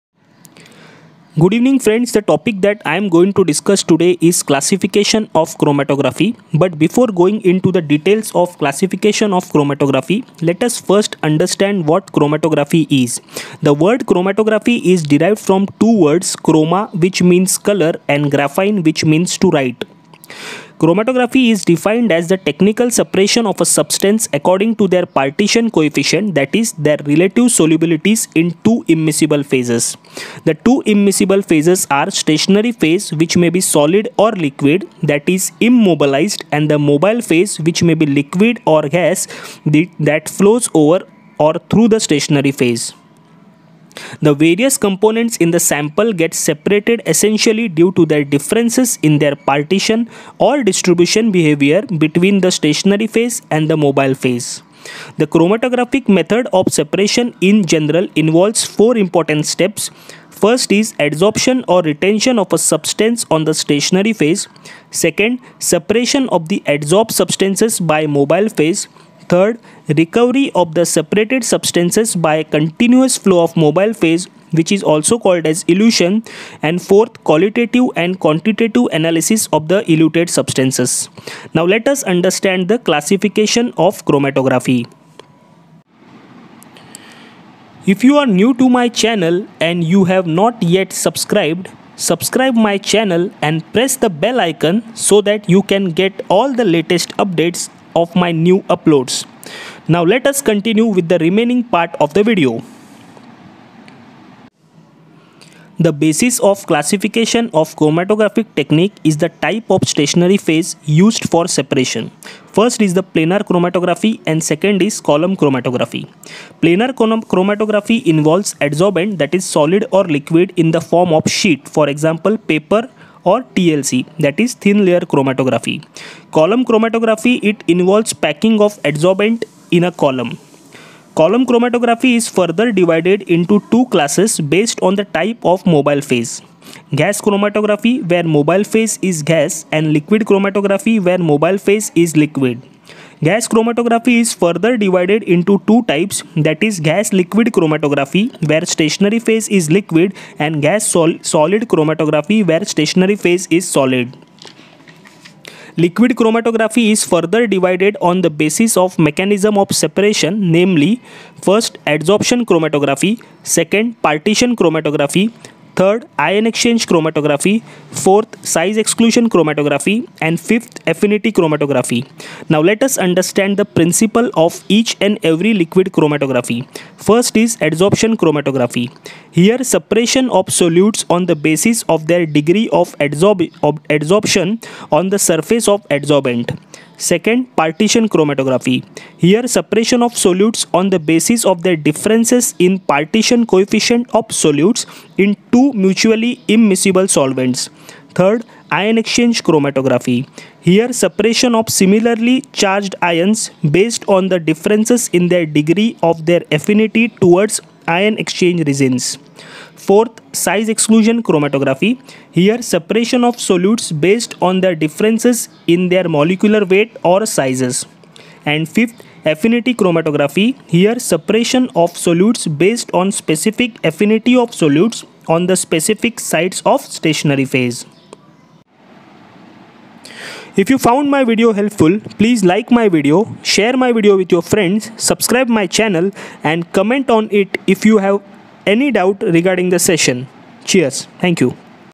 Types of Chromatography
This video describes broad Classification of Chromatography in detail